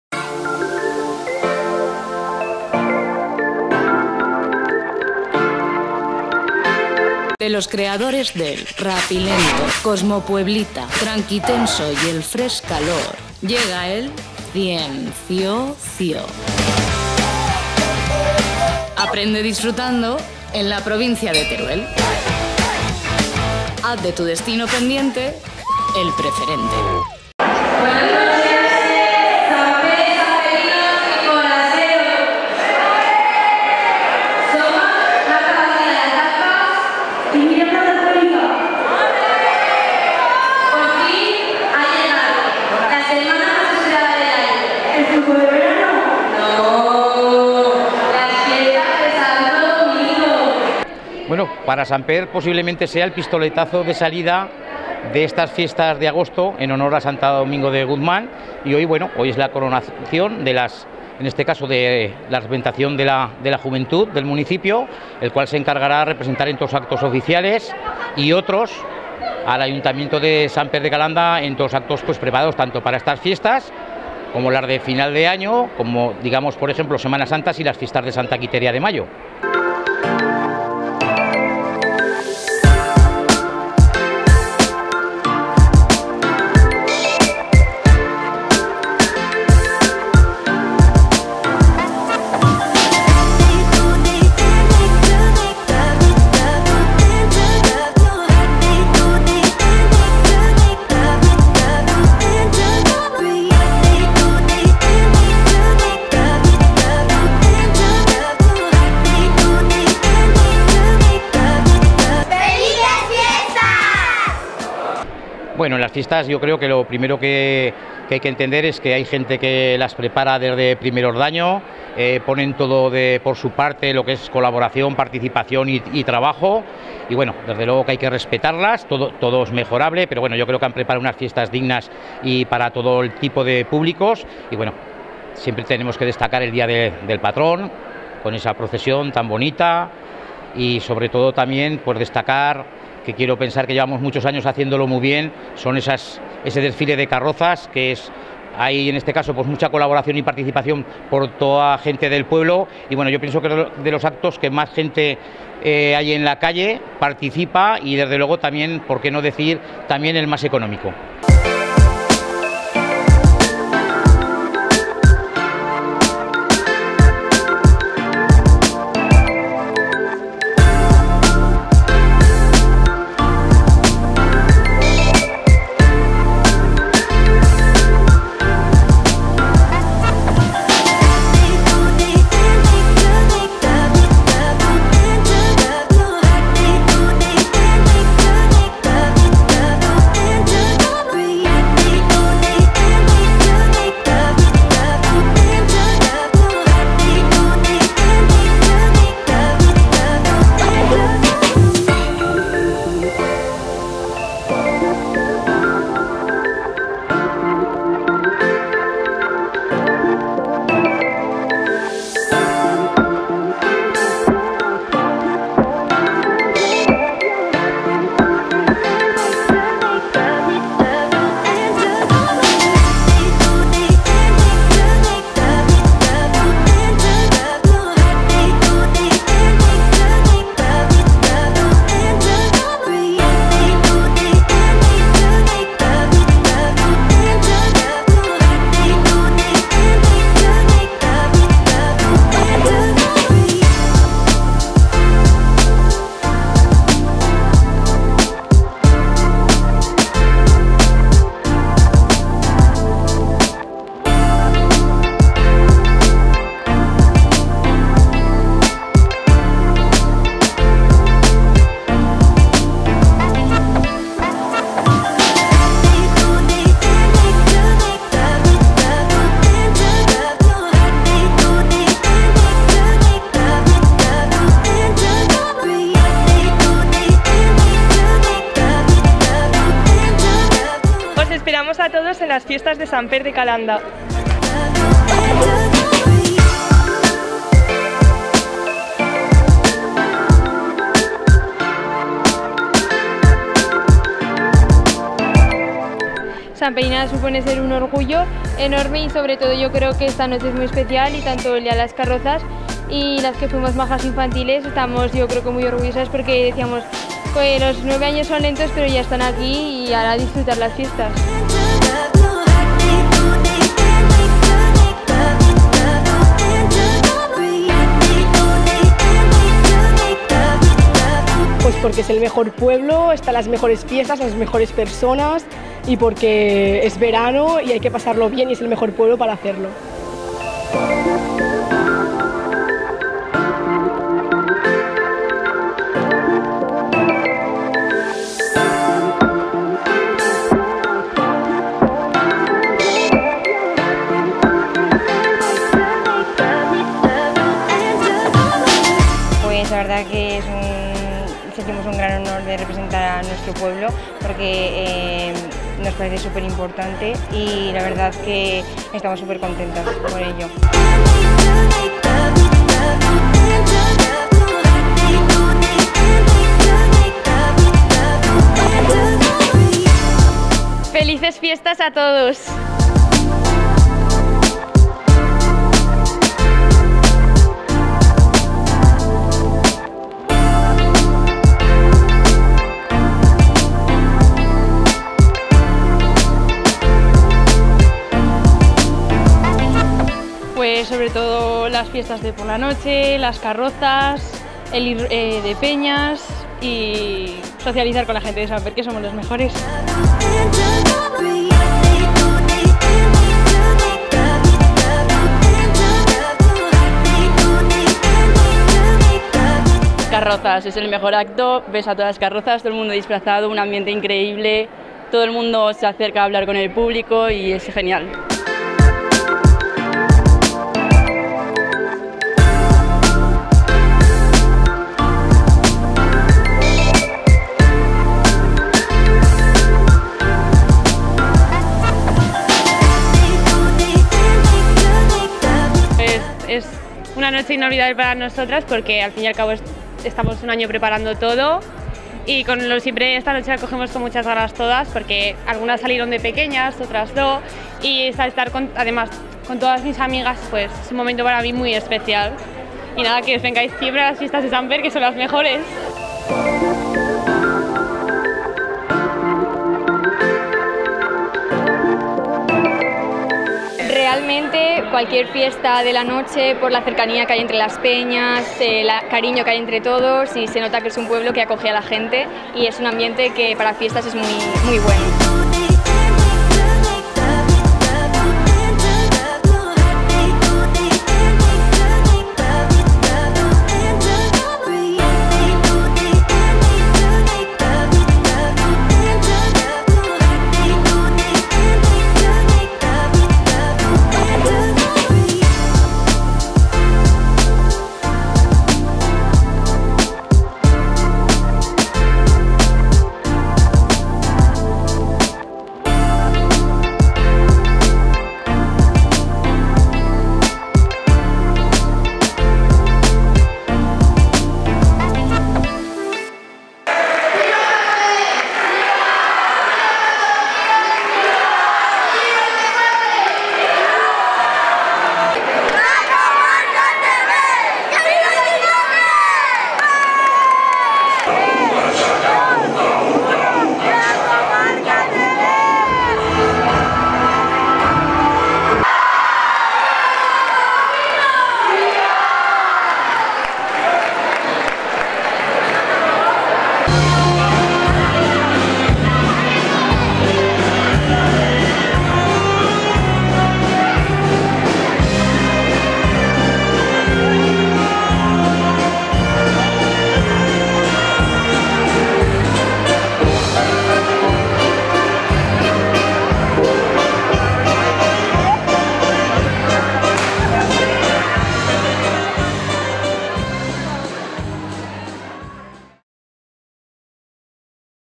Presentación de la MAJAS de SAMPER DE CALANDA | Fiestas 2023
Samper de Calanda vivió su noche más especial con la presentación de las Majas y Majicas de fiestas 2023. Una noche de emociones y abrazos. Tras ser recibidas en el ayuntamiento las jóvenes desfilaron hasta el pabellón de fiestas donde fueron presentadas y coronadas. El pistoletazo de salida ya se ha dado y como dijo el alcalde de la localidad, Alfonso Pérez, se esperan días de diversión y sobre todo mucho alegría.